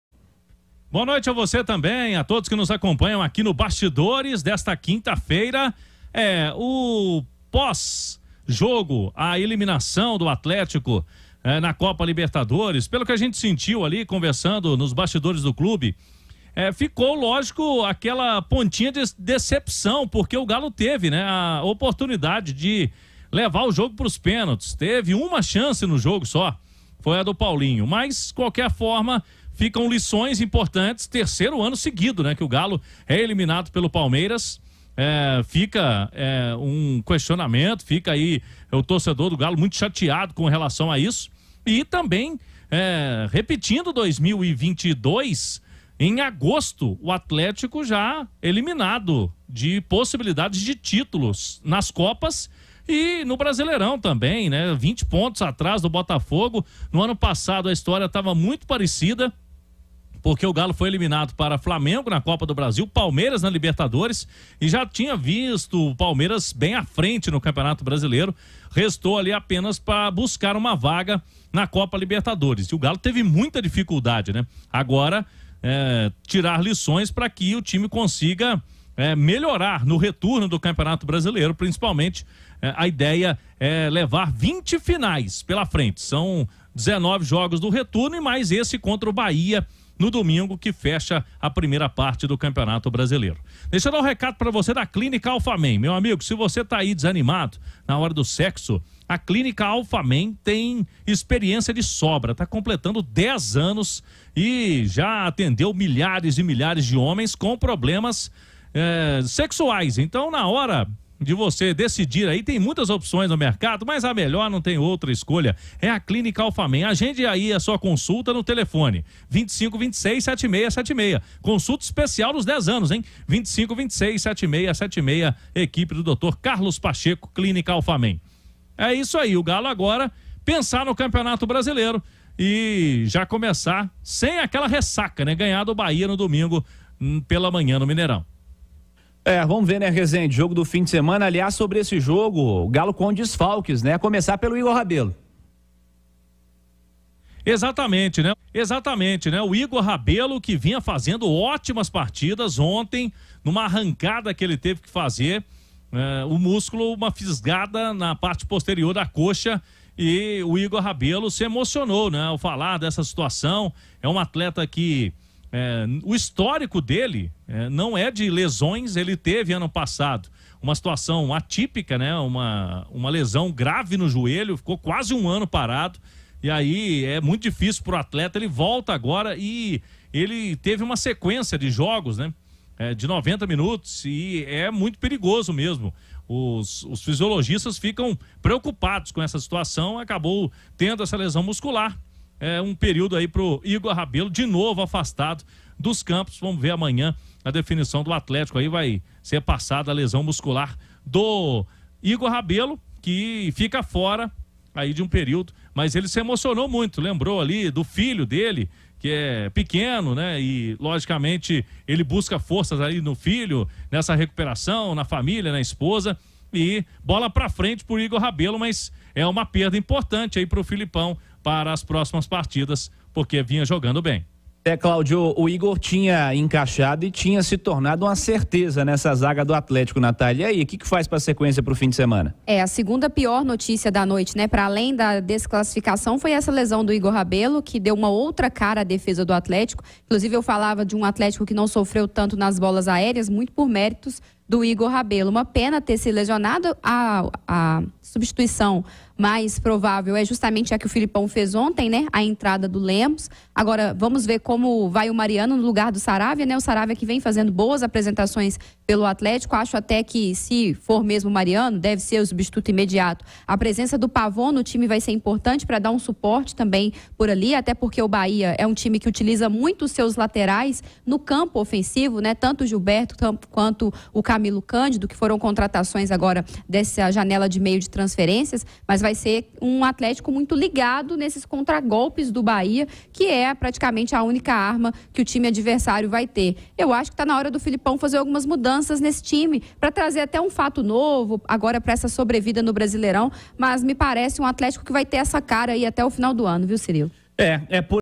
O DIA APÓS A ELIMINAÇÃO DO ATLÉTICO! QUAL A PROJEÇÃO DO GALO ATÉ O FIM DA TEMPORADA?
Claudio Rezende traz informações sobre os bastidores do Atlético após eliminação